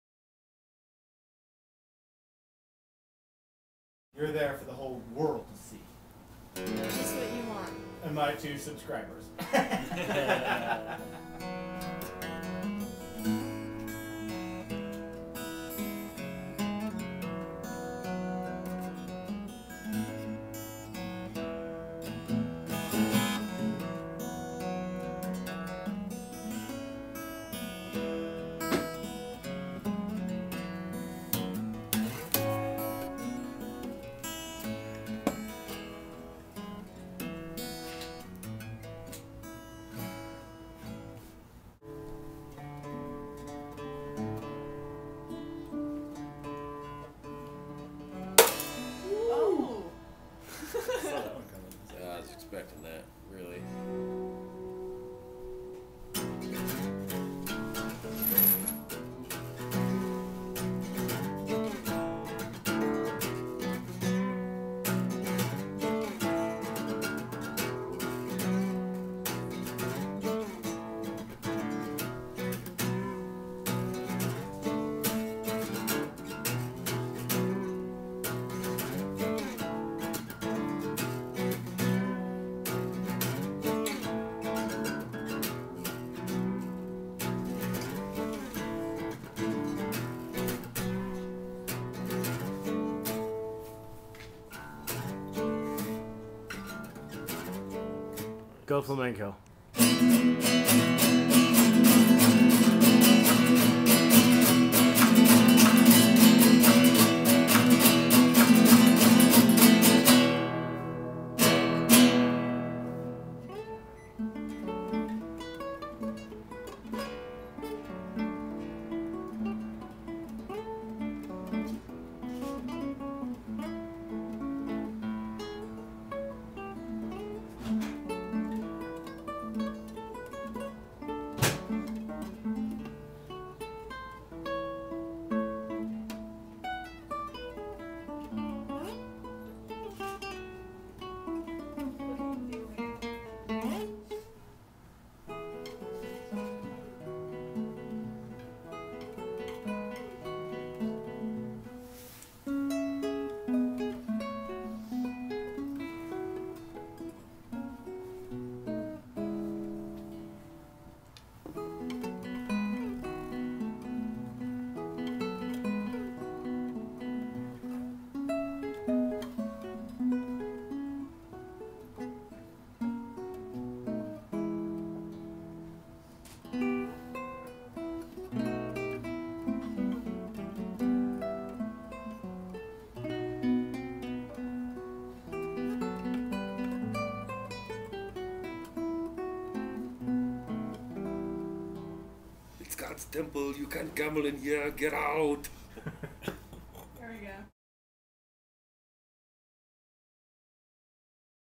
Tim & Jarad Guitar
Hangin out over the weekend 3-14-09, Variety and by the way they are much better than this video would indicate